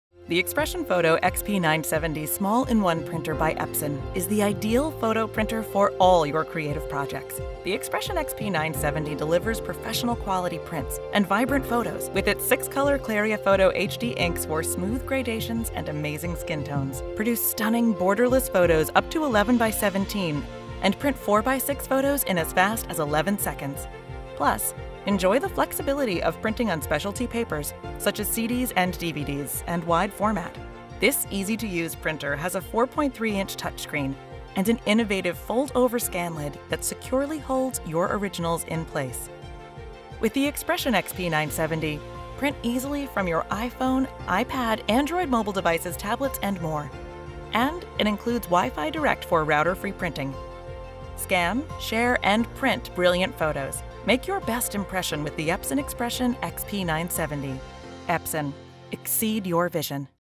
Epson Expression Photo XP-970 | Take the Tour
The Epson Expression Photo XP-970 Small-in-One Printer is the ideal photo printer for all of your creative projects. Learn more about the printer and all its features, including delivery of professional-quality printers with 6 HD inks, photo scanning, borderless photos, and mobile printing options.